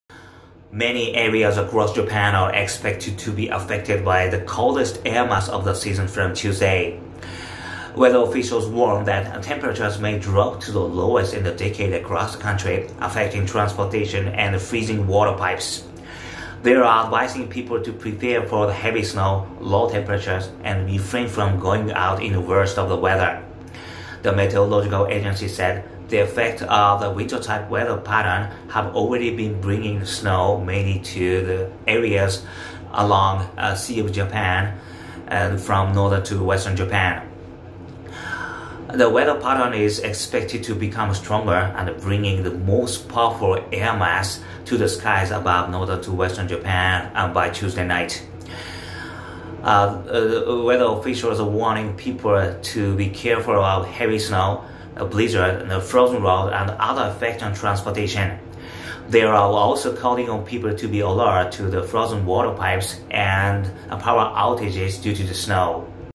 【Yoichi zig-zag news 23/1/25】Coldest air mass brings heavy snow and low temperatures to Japan
Weather officials are warning people to be careful of heavy snow, blizzard, frozen road and other effects on transportation, and also calling on people to be alert to frozen waterpipes and power outrages